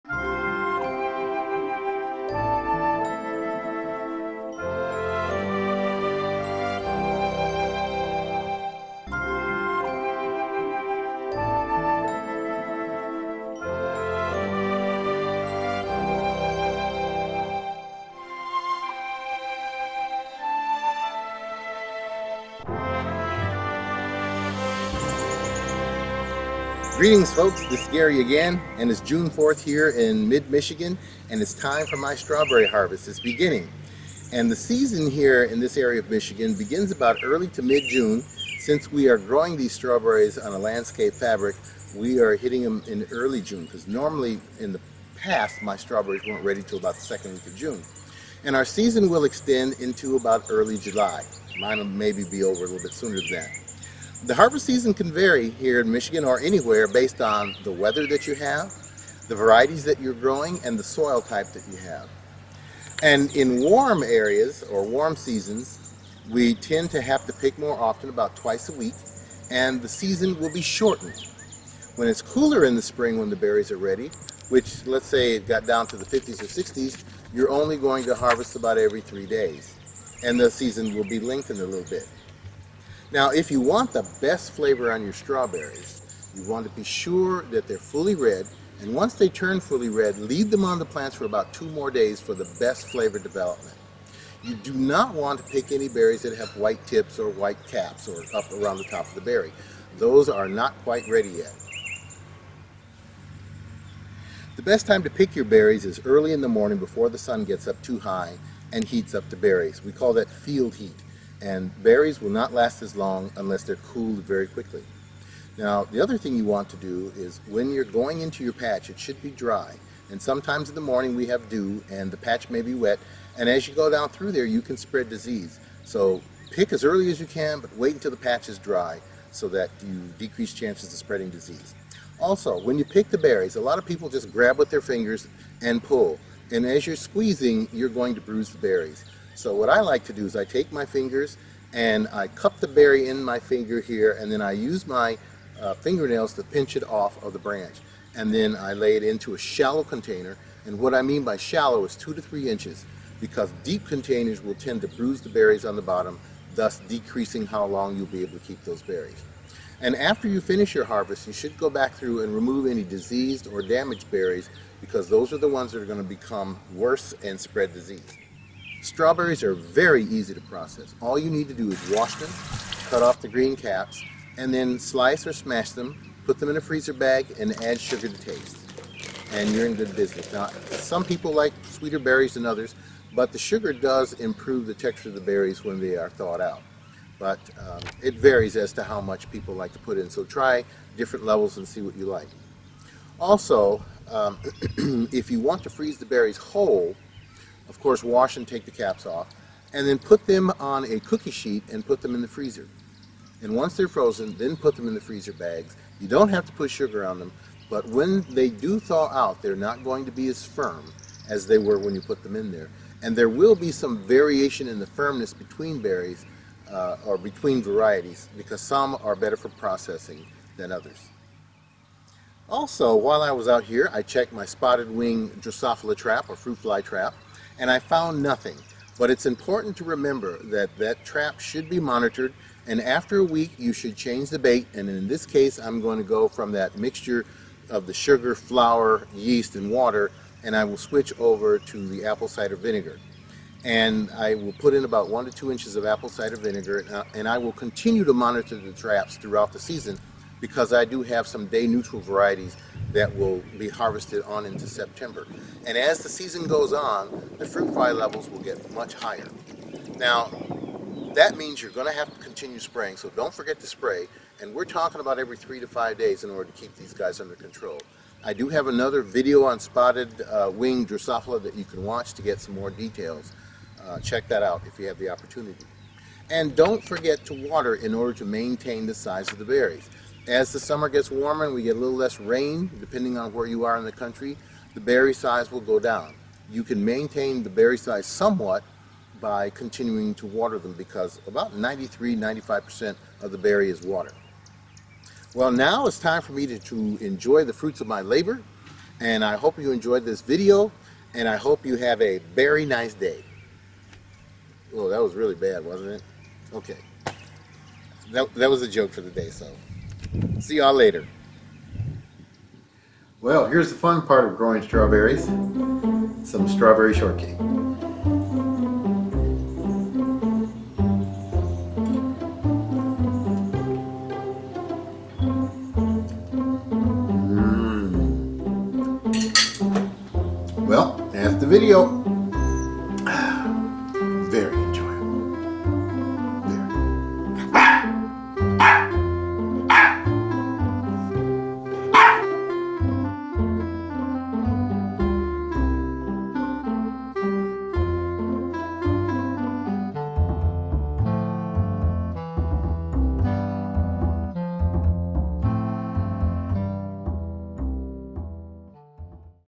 Strawberry Harvesting Storage and Processing
Description: Gary Heilig, retired Horticulture Educator for Michigan State University Extension discusses how to harvest, store and process home grown strawberries